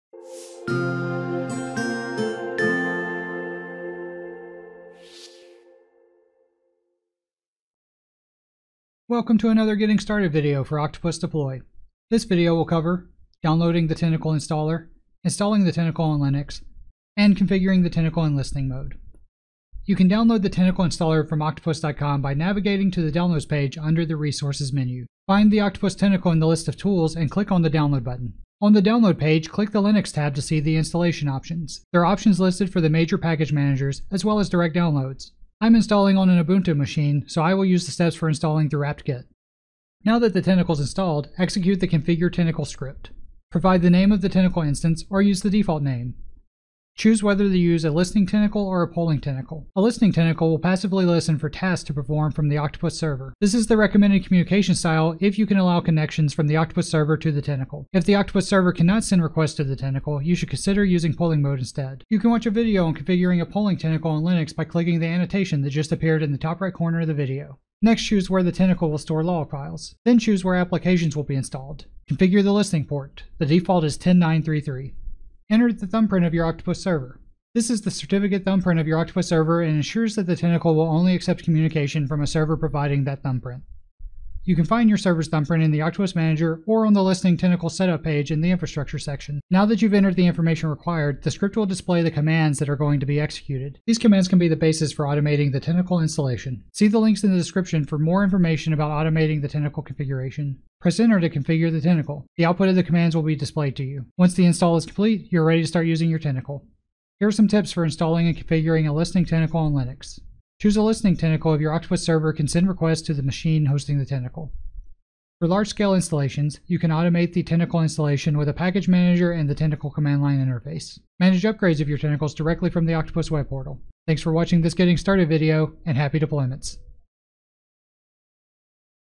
Getting Started - Installing a Listening Tentacle on Linux
This video will walk through installing and configuring a Listening Tentacle on Linux.